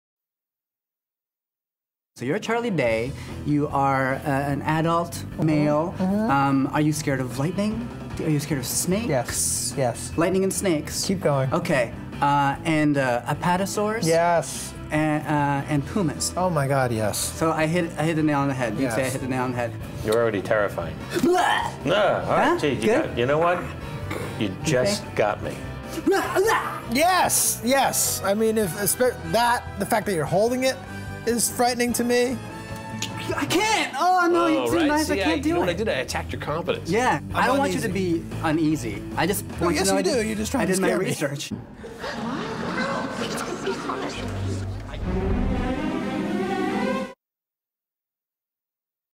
Big Fun Movies - Carlos asks the stars of Monsters University what scares them!
Carlos asks Monsters University stars Charlie Day, Billy Crystal and Nathan Fillion what scares them the most!